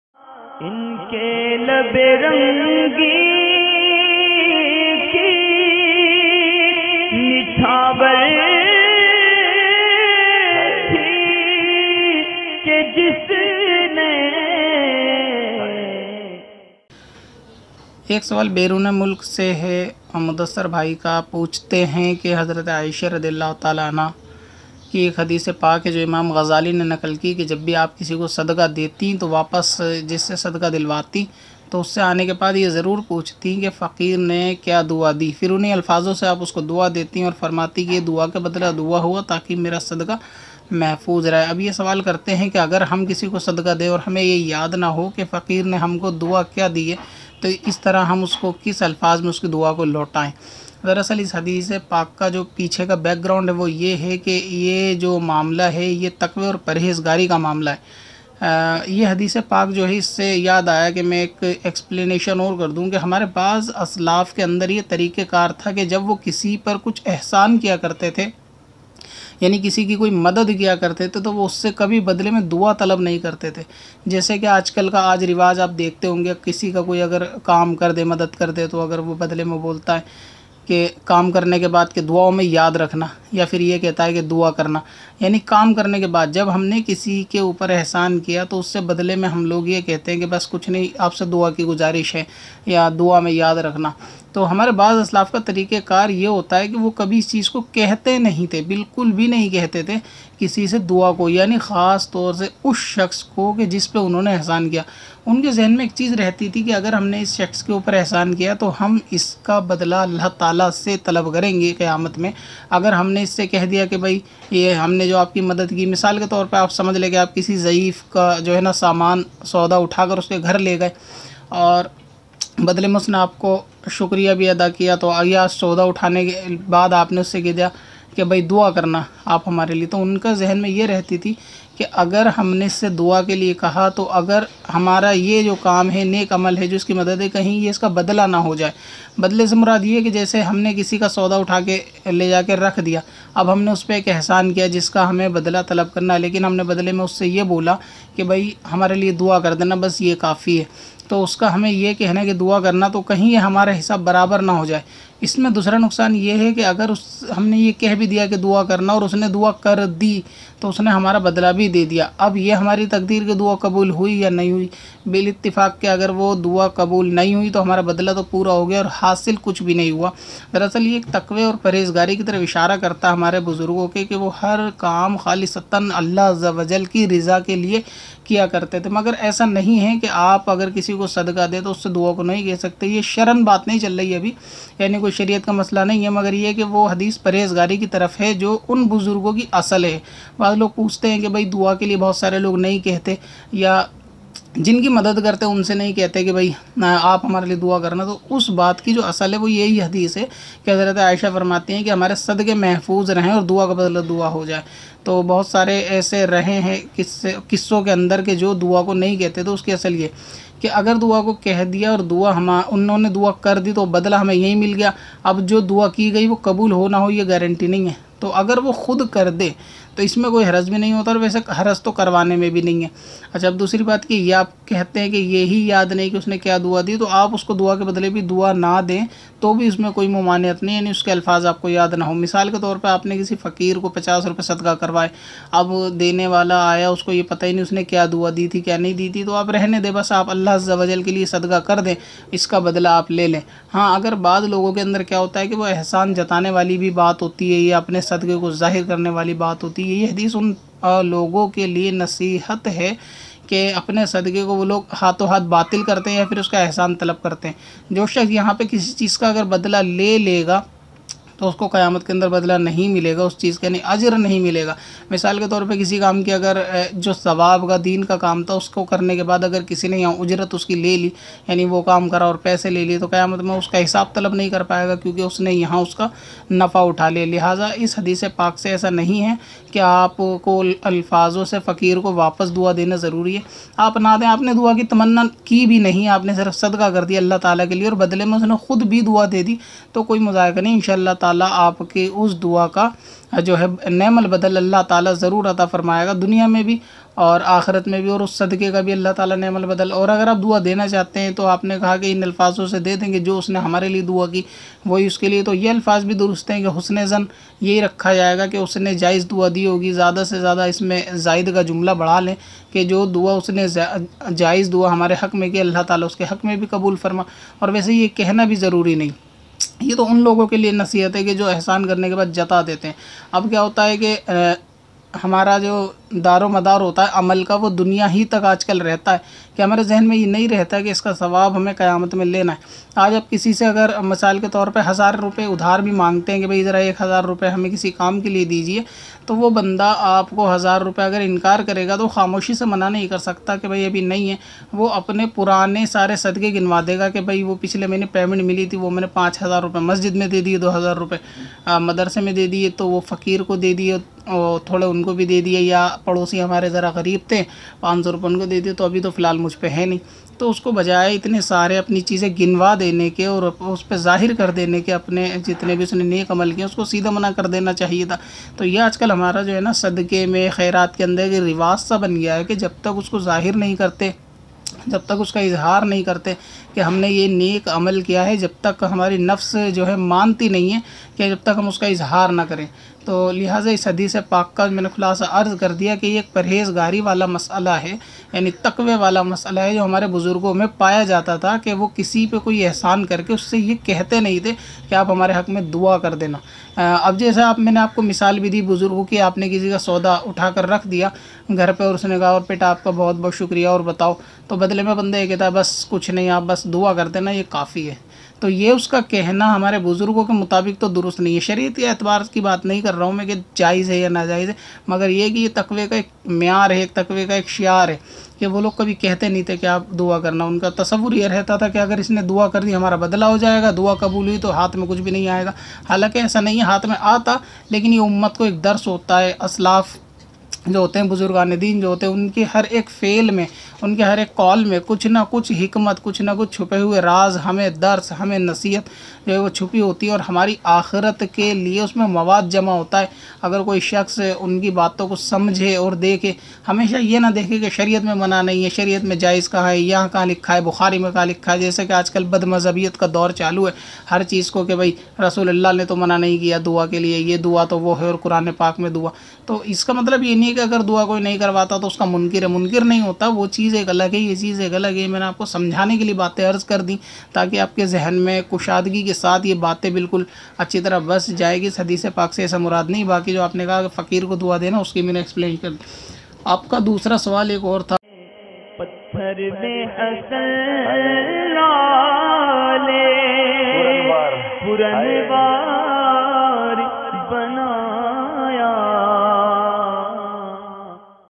अंदाज़े सूफियाना | Sadqa Dete Waqt Dua Ki Arz Karna Kaisa | Ek Hadis e Pak Ka Khulasa
(NEWS & Update) 📰 📅⏰
Aane Wali Videos & Live Timing Dekhen
kya kisi ko sadqa karte waqt dua ko keh sakte hai, kya kisi ki madad ke bad dua ki arz kar sakte hai. kya doosri se dua karwani chahiye
सदक़े के बाद दुआ के लिए कहना कैसा है,